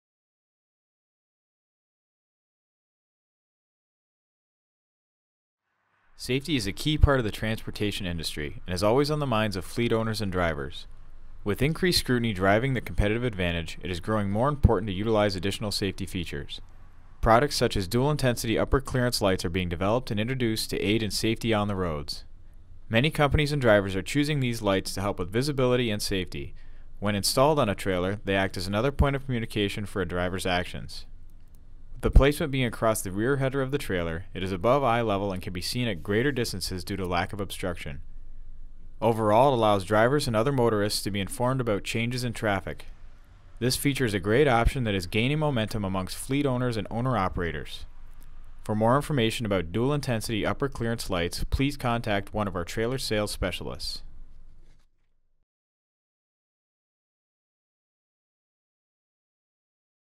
Dual Intensity Upper Clearance Lights
Safety is a key part of the transportation industry.  This video features safety options that are available on refrigerated or dry van trailers.  The benefits of these lighting systems are seen by many and increase safety on the roads.  Not only are these lights visible to Trucks on the road, but other motorists as well.  For more information about these lights and other options, please contact as Trailer Sales Specialist.